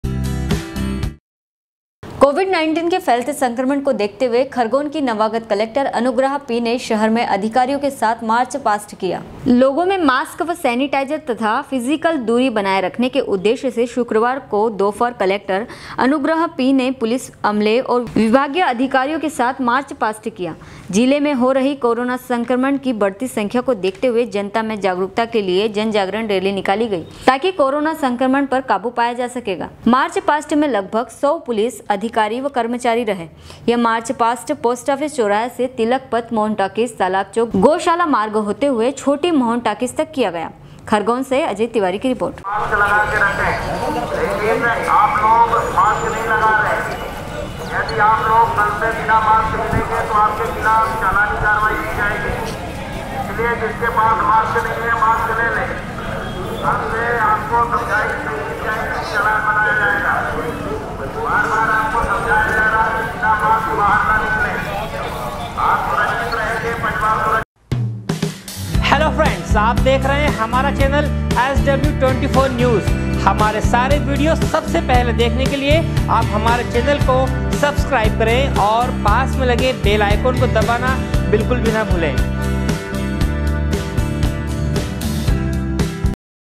SW 24 News Madhyapradesh
कोविड-19 के फैलते संक्रमण को देखते हुए, खरगोन की नवागत कलेक्टर अनुग्रहा पी ने शहर मे अधिकरीयो के साथ मार्च पास्ट किया। खरगोन से अजय तिवारी की रिपोर्ट